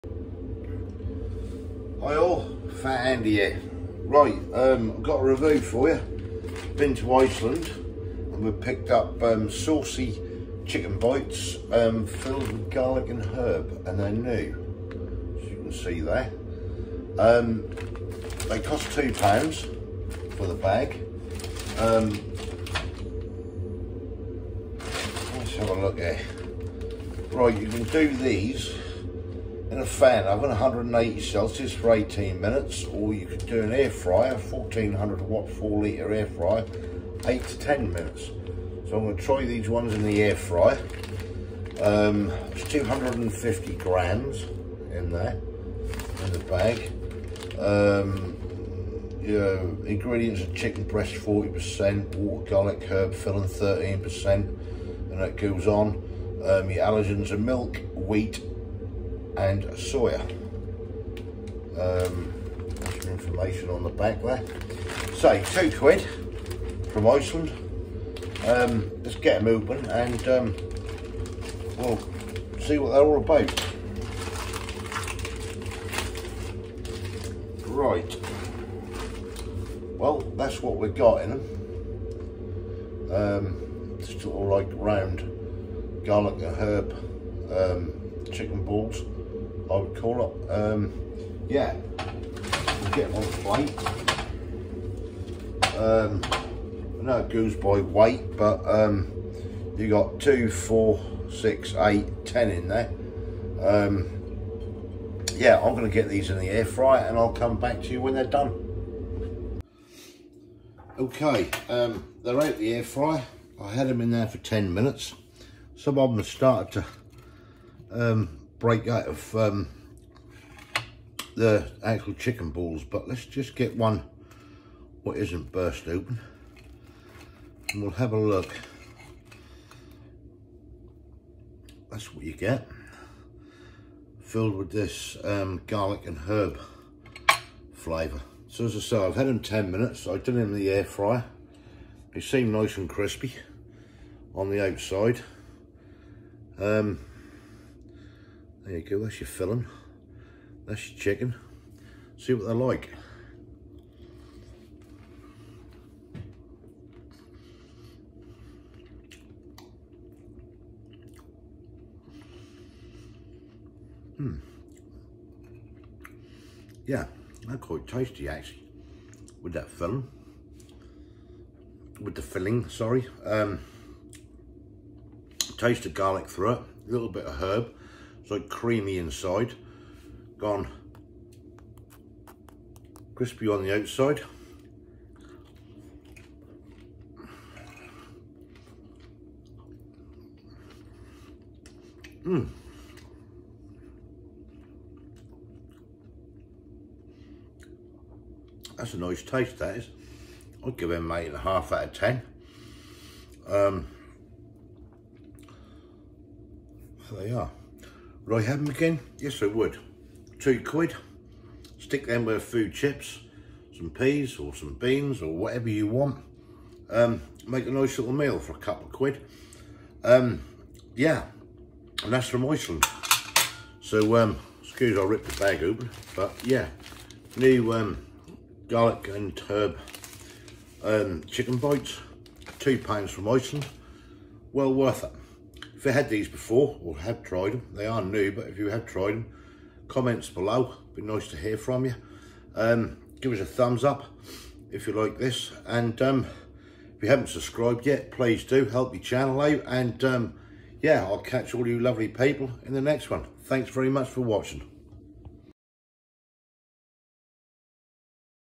New Chicken Bites, Garlic & Herb Sauce
Garlic & Herb chicken bites, new from Iceland £2 very nice worth a try